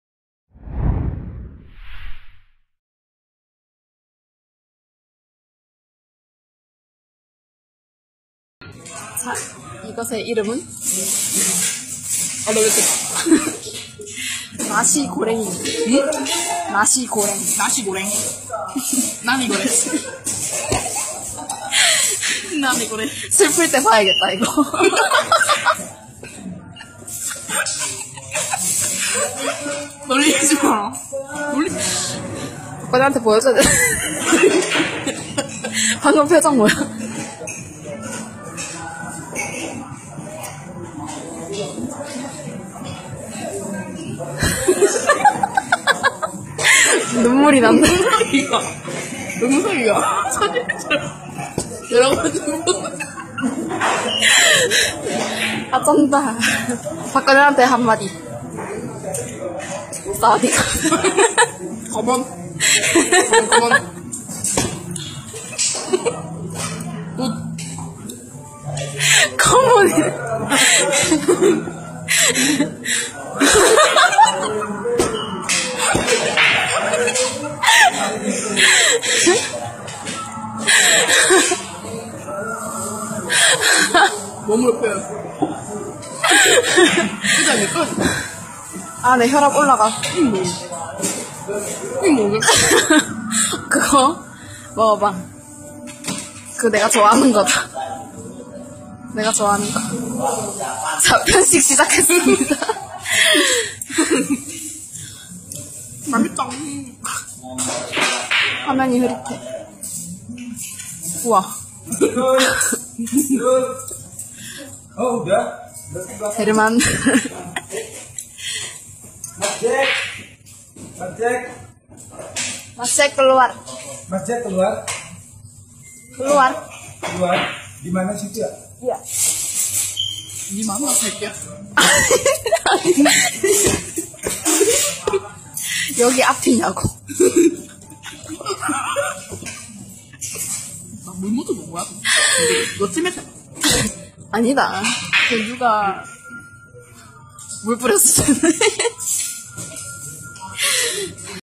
Reaksi Gadis Korea Pertama Kali Makan Nasi Goreng Di Restoran Indonesia di Kota Gimhae
Gadis Asal Korea ini adalah seorang pelajar setara SMA di Korea Selatan. Si Doi sedang mencicipi masakan khas Indonesia "Nasi Goreng" disalah satu Warung Indonesia  "KRAKATAU"Di  kota Gimhae.